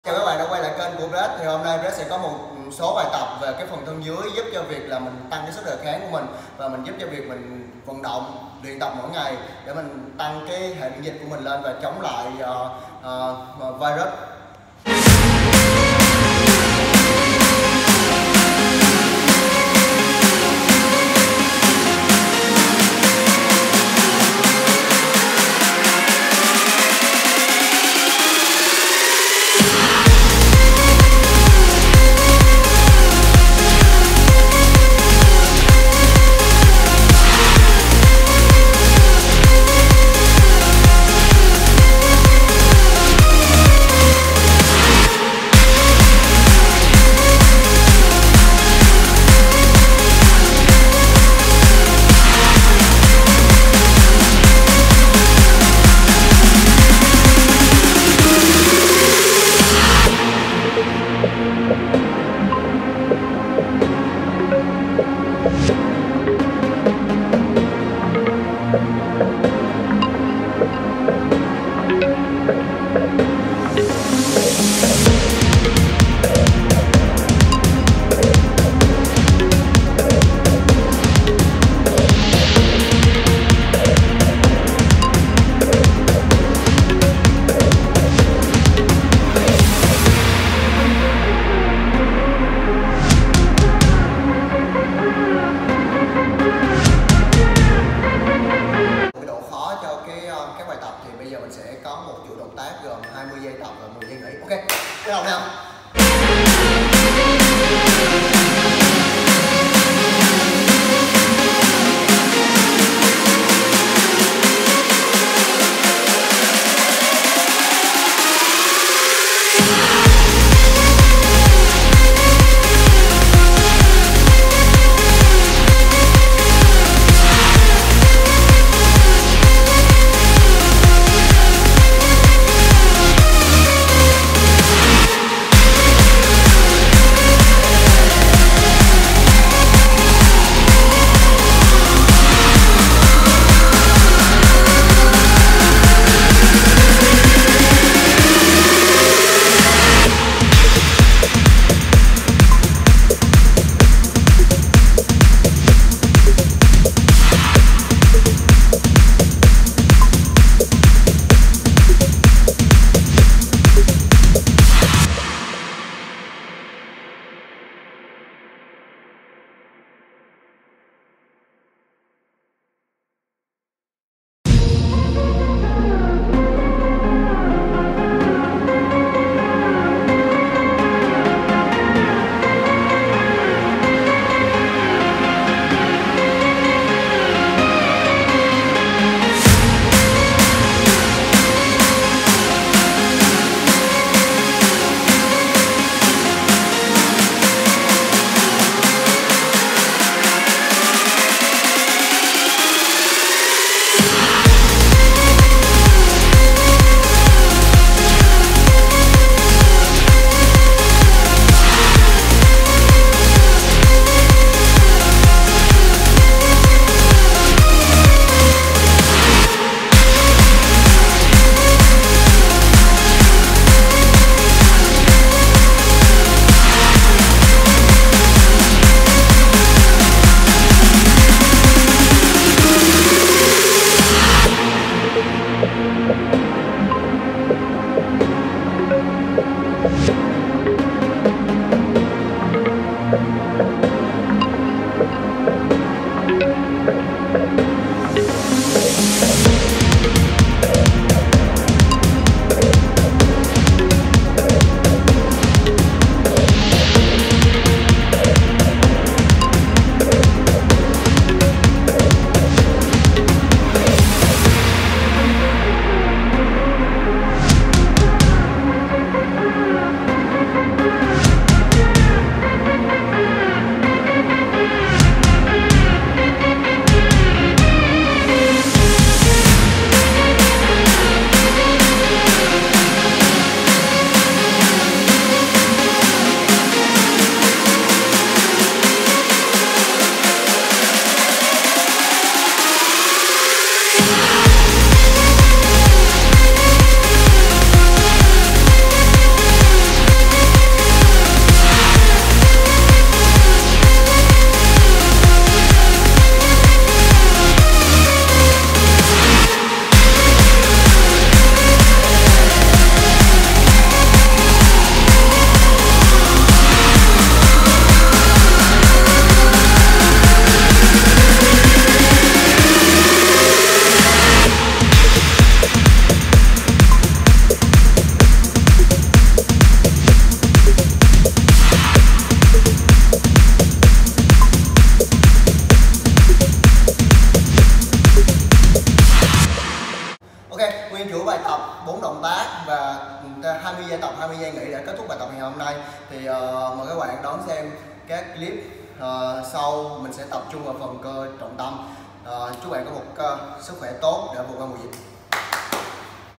Những bài tập sử dụng trọng lượng cơ thể, giúp tăng cường hệ miễn dịch.
Luyện tập thường xuyên + chế độ dinh dưỡng sẽ tăng cường hệ miễn dịch . Trong video này Brad sẽ chia sẽ các bài tập mà mọi người có thể tự tập ở nhà trong giai đoạn này.